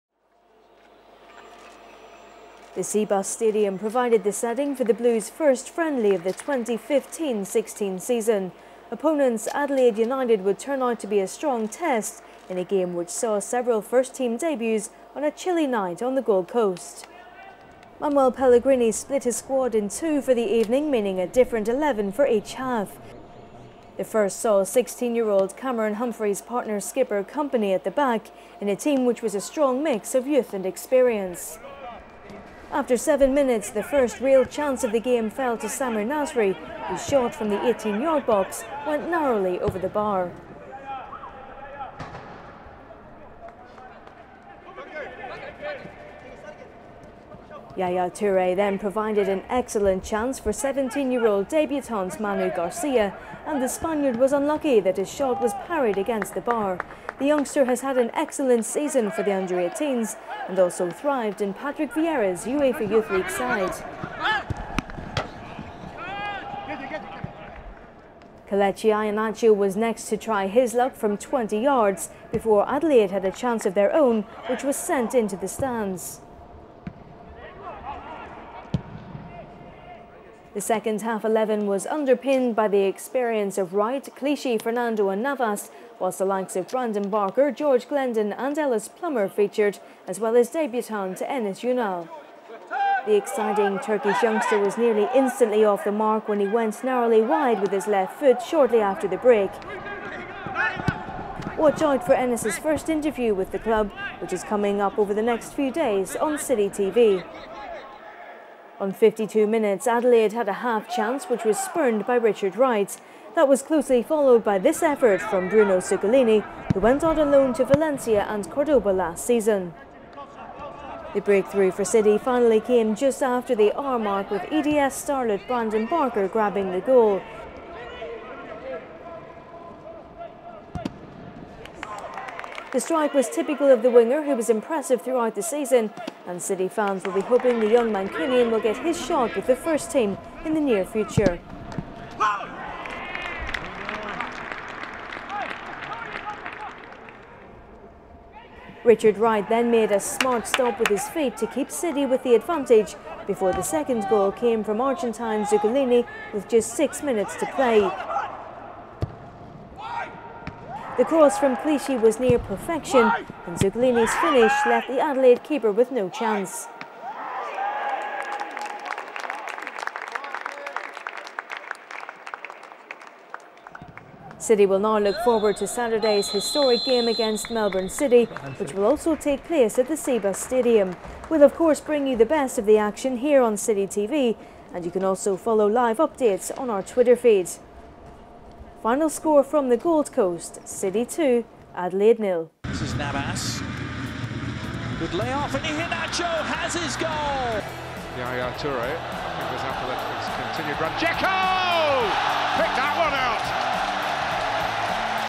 EXCLUSIVE MATCH HIGHLIGHTS | Man City 2-0 Adelaide
Watch exclusive match highlights from Man City's first pre-season game during their tour of Australia. Goals came from Brandon Barker and Bruno Zucculini.